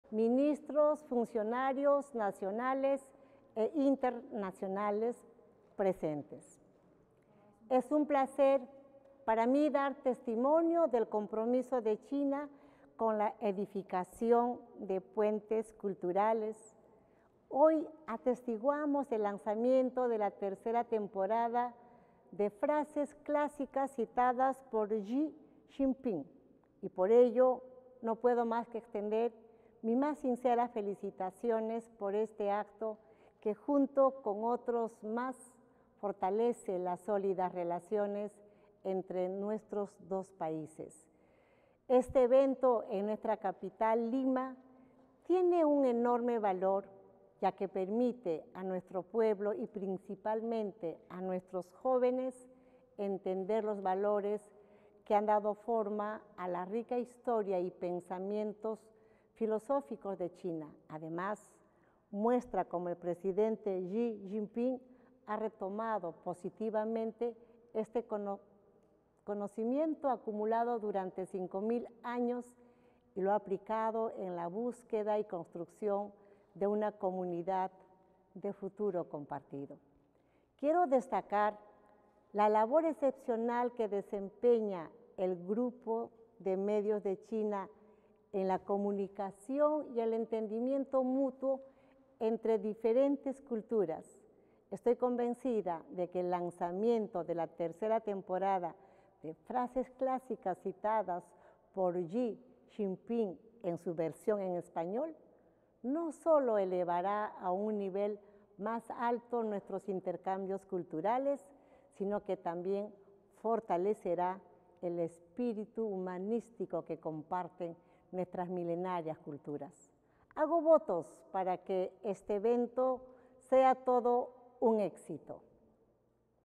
La presidenta peruana felicitó la emisión de la serie “Frases Clásicas Citadas por Xi Jinping”
Con motivo de la visita de Estado del presidente chino, Xi Jinping, a Lima para asistir a la 31ª Reunión de Líderes Económicos del APEC, invitado por la presidenta peruana, Dina Boluarte, se ha lanzado la tercera temporada (versión en español) de la serie "Frases Clásicas Citadas por Xi Jinping", producida por el Grupo de Medios de China (CMG, por sus siglas en inglés). La serie fue emitida en varios medios de comunicación de Perú. La presidenta peruana, Dina Boluarte, envió un mensaje de vídeo para felicitar la emisión del programa, destacando que la versión en español no solo elevará los intercambios culturales a un nivel superior, sino que también fortalecerá el espíritu humanístico compartido por estas milenarias culturas.